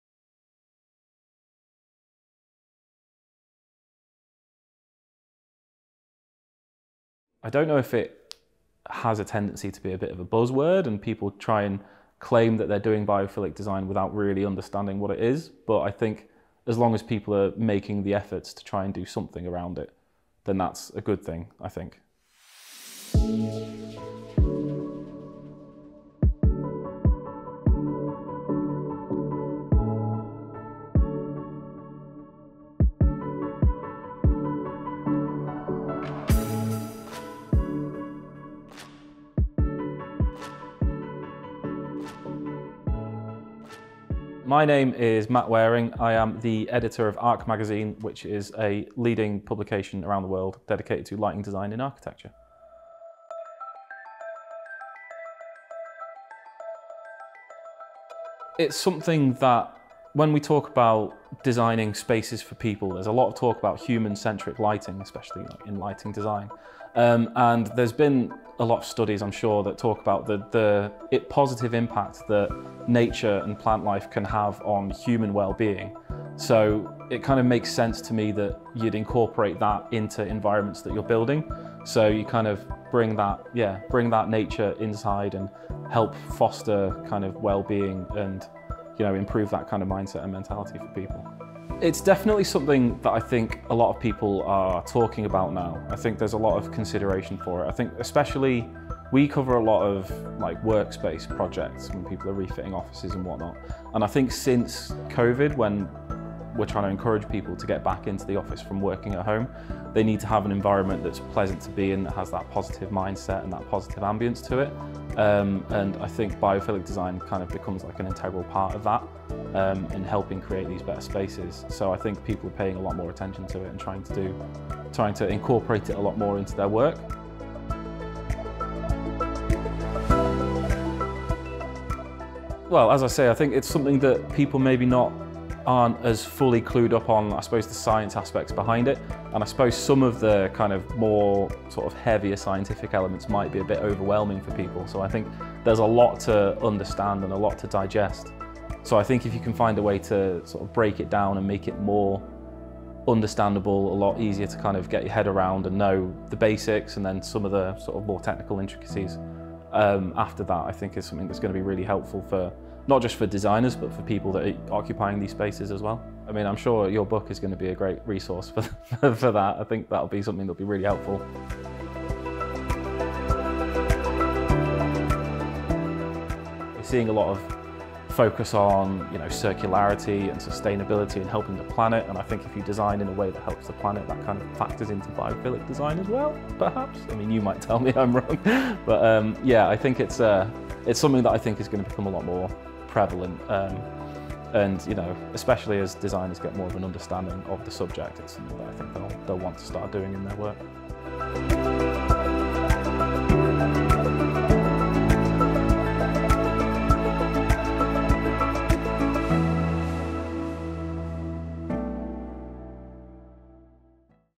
Matt Waring, Editor Arc Magazine - "The Light Within Us"
Whether you're an architect, designer, or simply passionate about sustainable living, "The Light Within Us" and Matt's insights offer valuable knowledge and practical solutions for creating harmonious and healthy living spaces that improve people's lives and well-being.